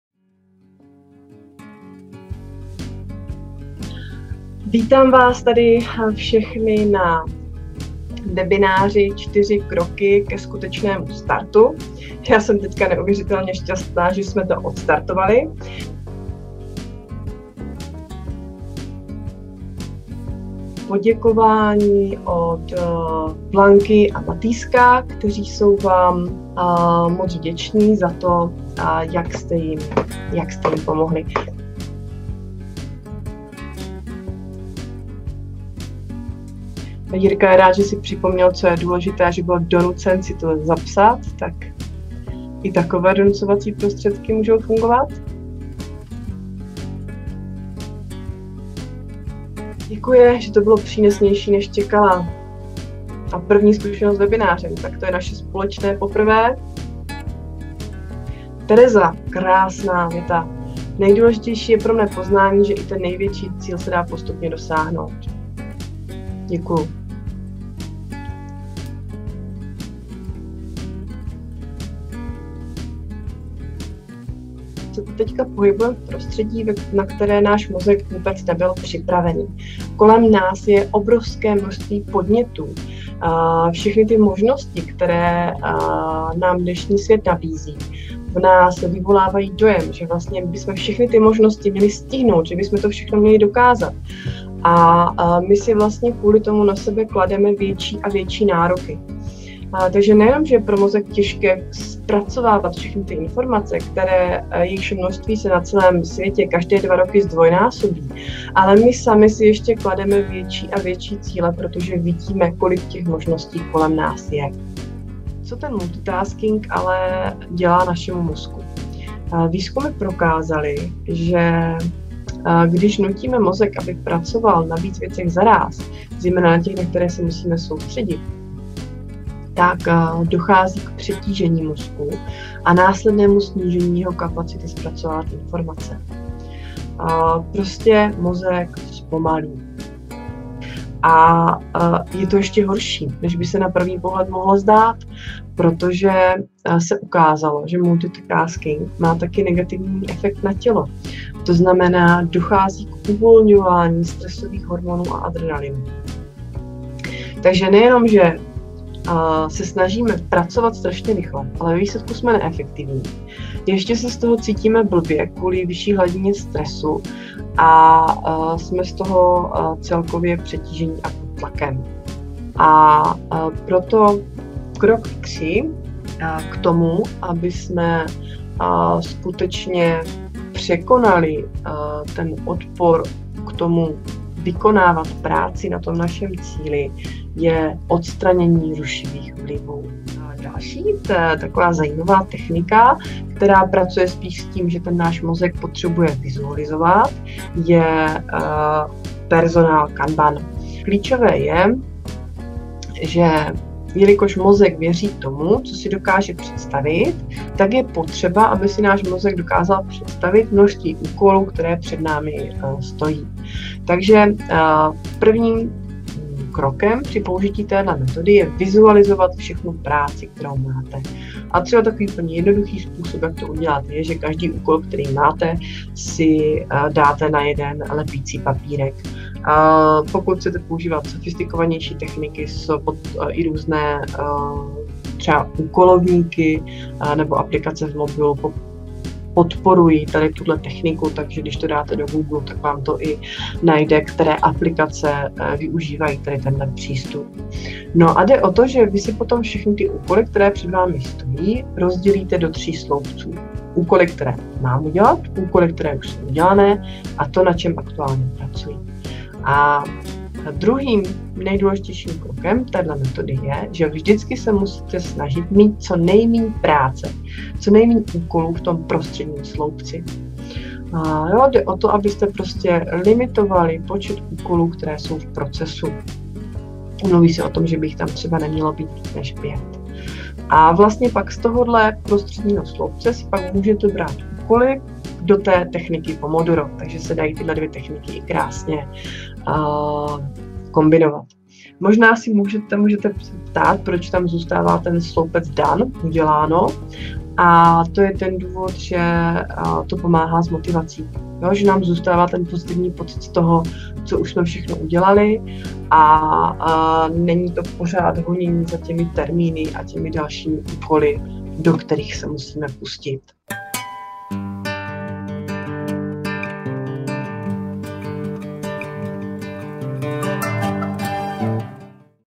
Webinář Čtyři kroky ke skutečnému startu - ukázka (multitasking, osobní kanban)
Koukněte za oponu webináře. A můžete také zjistit jak (ne)funguje multitasking a jak nad ním můžete zvítězit pomocí techniky nazvané osobní kanban. Detailnější popis techniky naleznete zde